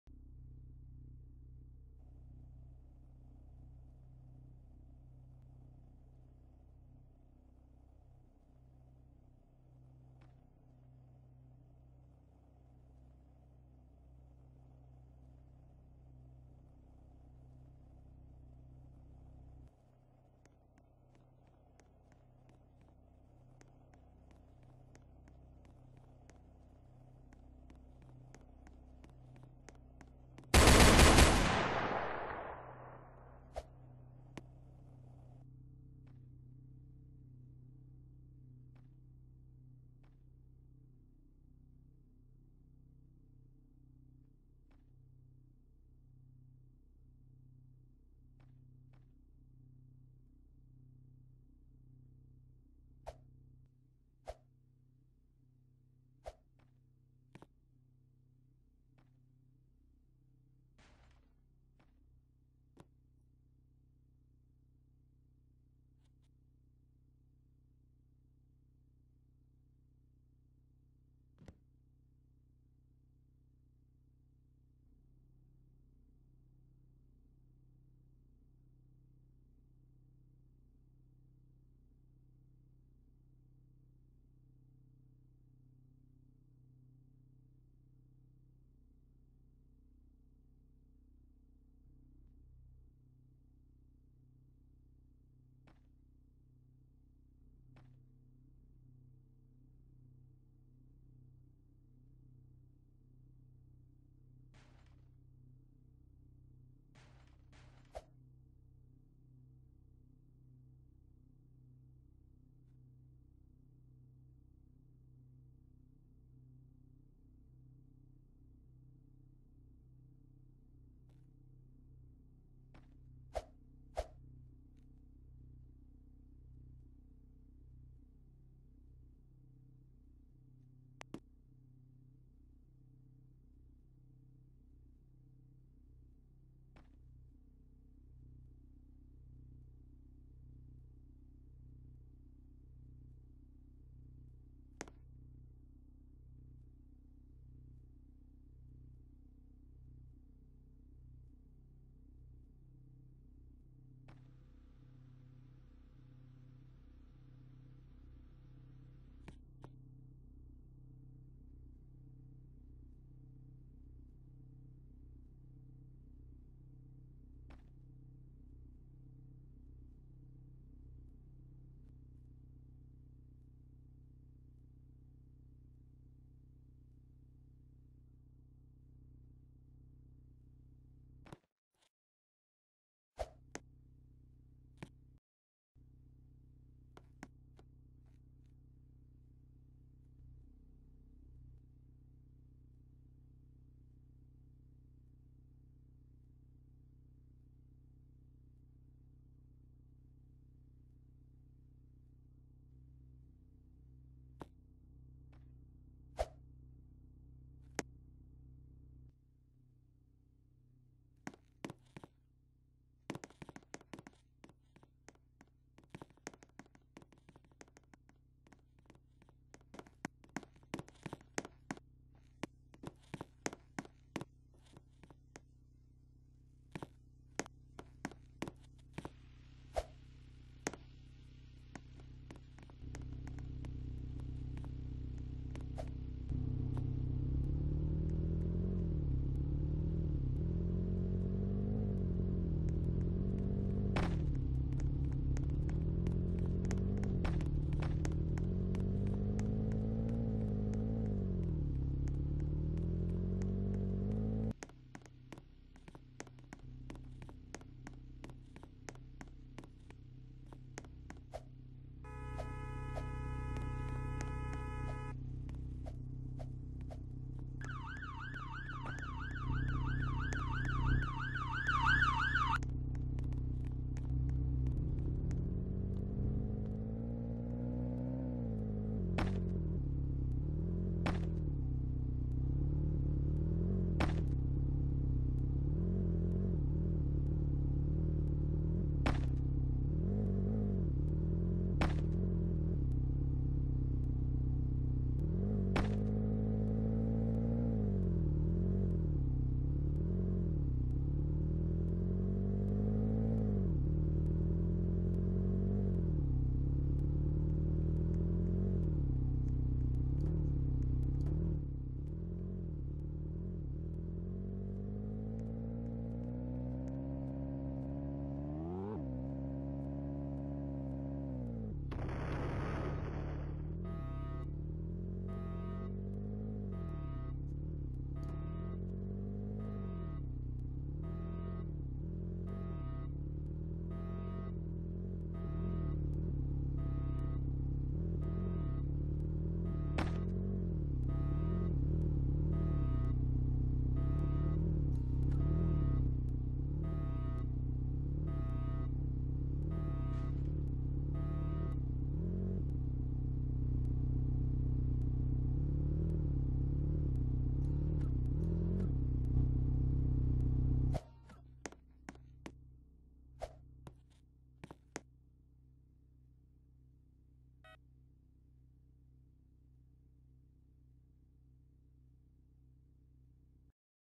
The Story of Military VS Edd , Gaster and Zander | Part 3/3 | SCP Chopping a guy leg!
Watch till the end to see that we discovered SCP